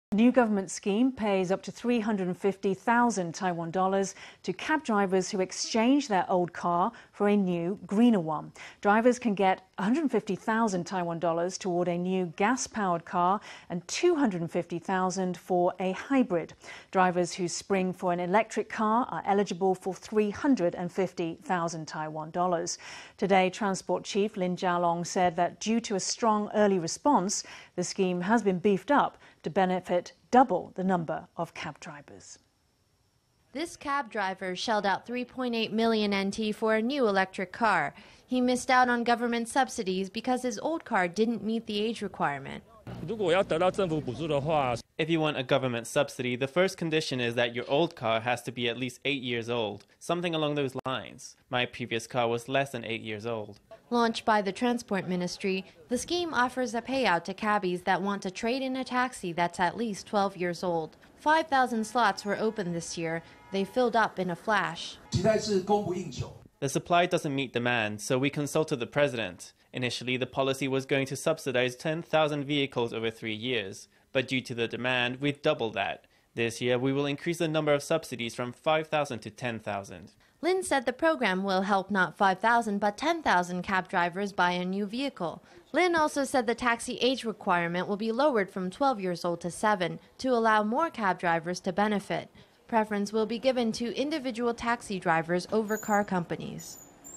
This year, 10,000 cab drivers to get up to NT$350,000 toward new taxi
A new government scheme pays up to NT $350,000 to cab drivers who exchange their old car for a new, greener one. Drivers can get NT $150,000 toward a new gas-powered car and NT$250,000 for a hybrid. Drivers who spring for an electric car are eligible for NT $350,000. Today transport chief Lin Chia-lung said that due to a strong early response, the scheme has been beefed up to benefit double the number of cab drivers.This cab driver shelled out NT$3.8 million for a new electric car. He missed out on government subsidies because his old car didn’t meet the age requirement.Mr. ChenTaxi driverIf you want a government subsidy, the first condition is that your old car has to be at least eight years old, something along those lines. My previous car was less than eight years old.Launched by the transport ministry, the scheme offers a payout to cabbies that want to trade in a taxi that’s at least 12 years old. Five thousand slots were opened this year. They filled up in a flash.Lin Chia-lungTransportation ministerThe supply doesn’t meet demand, so we consulted the president. Initially the policy was going to subsidize 10,000 vehicles over three years, but due to the demand, we’ve doubled that. This year, we will increase the number of subsidies from 5,000 to 10,000.Lin said the program will help not 5,000 but 10,000 cab drivers buy a new vehicle. Lin also said the taxi age requirement will be lowered from 12 to seven years old, to allow more cab drivers to benefit. Preference will be given to individual taxi drivers over car companies.